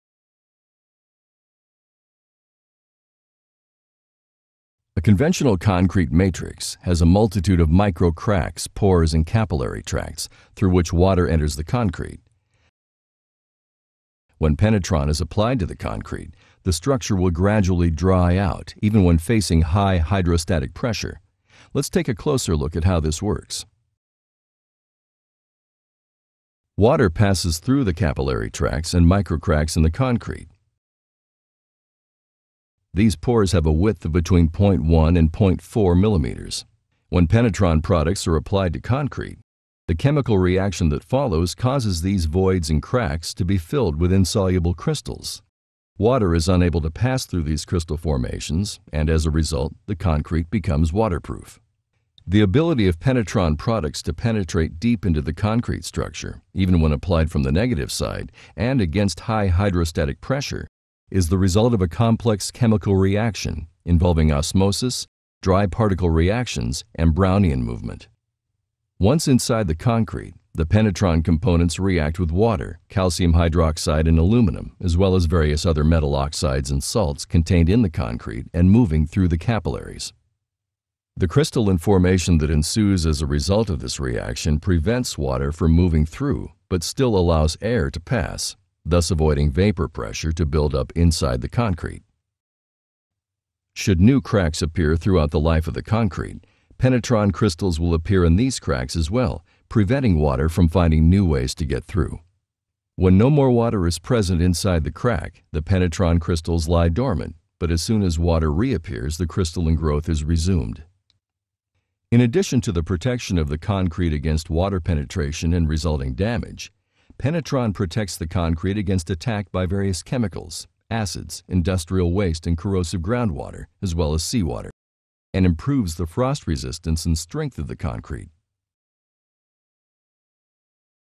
Penetron - How does it works.mpg
Penetron - Concrete waterproofing by crystallization

Penetron's waterproofing effect is achieved by the reaction of the various
chemical components contained in the solution when combined within the
concrete matrix. The compound penetrates deep into the capillary tracts of
the concrete by pressure of osmosis and forms crystals that completely
seal the capillaries and shrinkage cracks to drive out moisture. The
process works with or against the pressure of water. In the absence of
moisture, Penetron components lie dormant. Should moisture recur at any
time, the chemical action and sealing process repeats itself automatically and
advances even more deeply into the concrete. Penetron chemicals will continuously seal and reseal due to their chemical nature. Crystalline growth
from capillary waterproofing has been measured as deep as nearly 1 meter
from the point of application. Penetron® is 100% compatible with
concrete, brick, mortar, and stone.